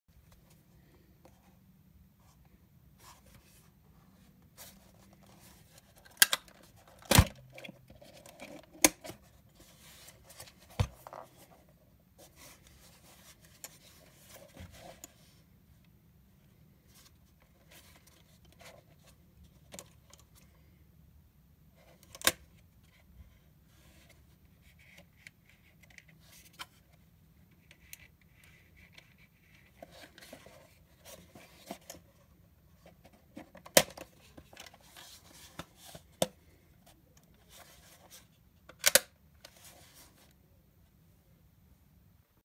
Star Wars Andor Prop Polaroid Camera
Polaroid camera transformed into a device that rebels use to navigate space travel.